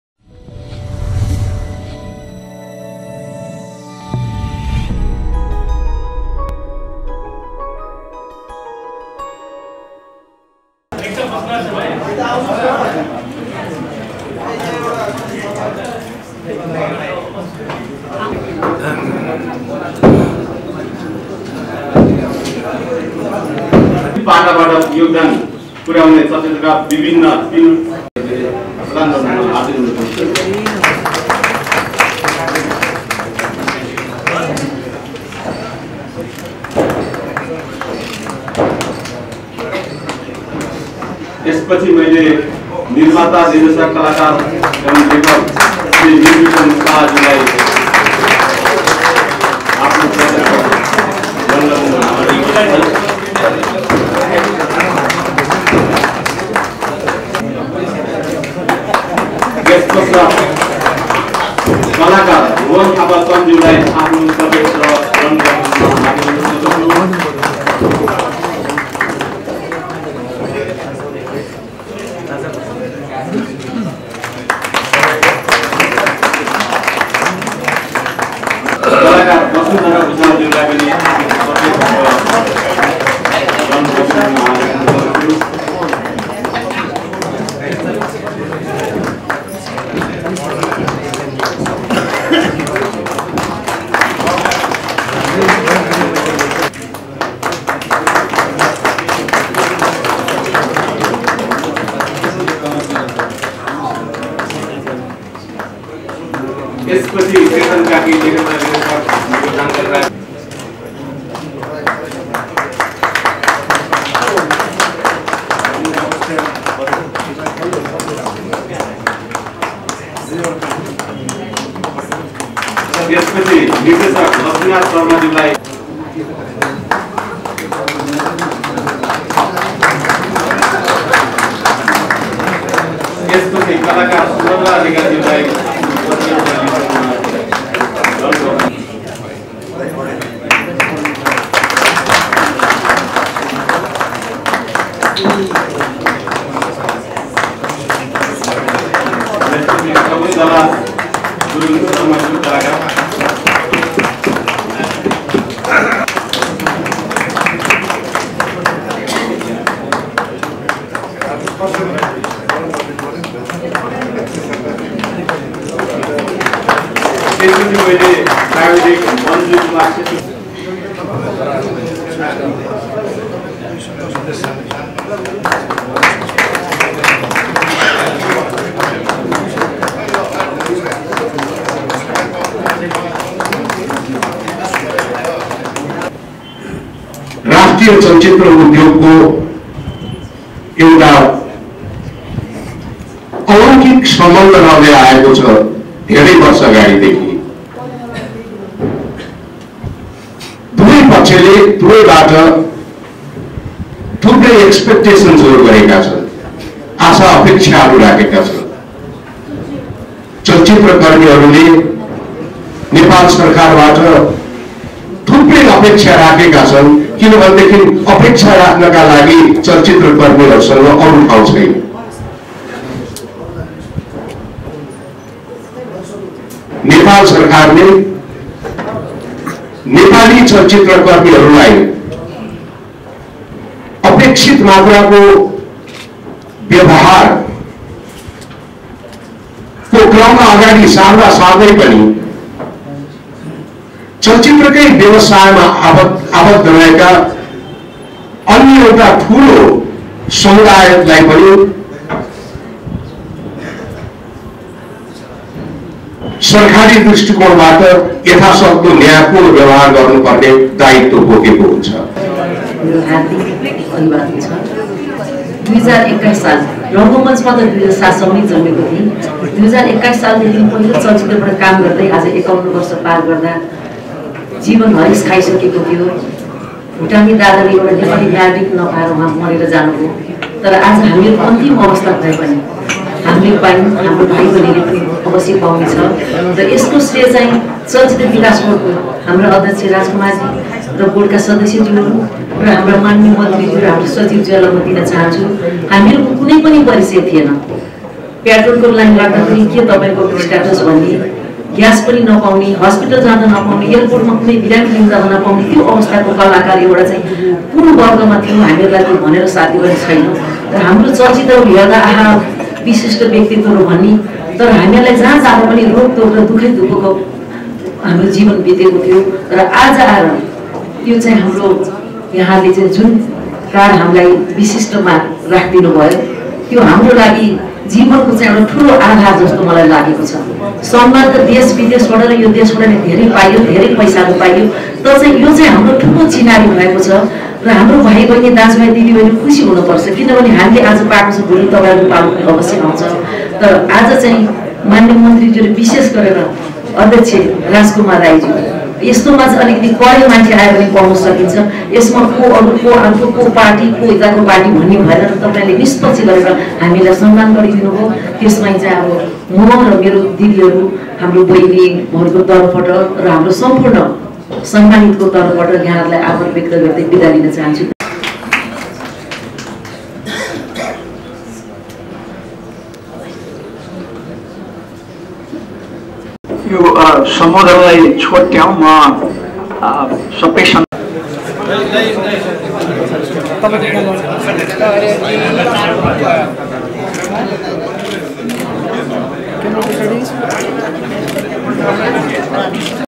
Nepali Artists receive appreciation certificate | Medianp.com
Medianp Presents to you exclusive footage from the  the Film Crew Certificate Distribution program.

During the program different film workers and artists receive certificate of appreciation.